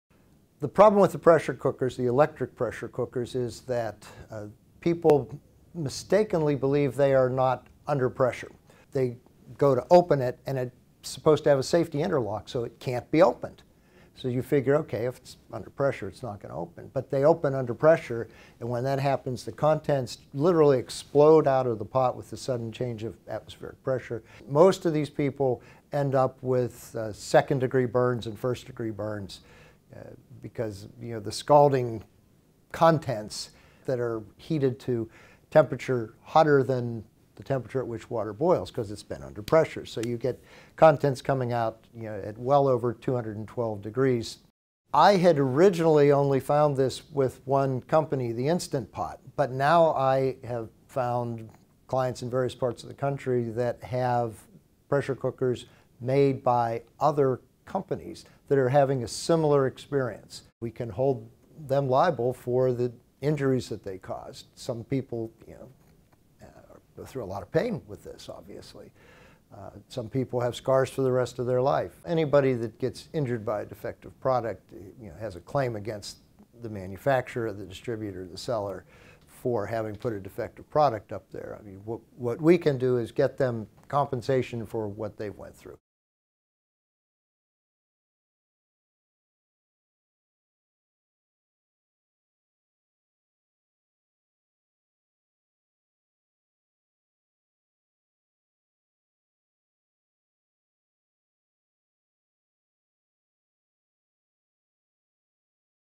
Are Pressure Cookers Safe? Users Report Explosions and Burn Injuries
Are pressure cookers safe to use? Many users have reported that the cookers can open while under intense pressure, leading to explosions and burn injuries.

George McLaughlin is a personal injury and product liability attorney who accepts cases from across the country. He is based in Denver, Colorado. In this video, he explains the problem with pressure cookers.

Video Transcript:

The problem with the pressure cookers, the electric pressure cookers is that people mistakenly believe they are not under pressure. They go to open it and it's supposed to have a safety interlock so it can't be opened. So you figure, OK, if it's under pressure, it's not going to open, but they open under pressure. And when that happens, the contents literally explode out of the pot with the sudden change of atmospheric pressure. Most of these people end up with second degree burns and first degree burns because the scalding contents that are heated to temperature hotter than the temperature at which water boils. Because it's been under pressure. So you get contents coming out at well over 212 degrees. I had originally only found this with one company, the Instant Pot. But now I have found clients in various parts of the country that have pressure cookers made by other companies that are having a similar experience. We can hold them liable for the injuries that they caused. Some people go through a lot of pain with this. Obviously, some people have scars for the rest of their life. Anybody that gets injured by a defective product has a claim against the manufacturer or the distributor or the seller for having put a defective product up there. What we can do is get them compensation for what they went through.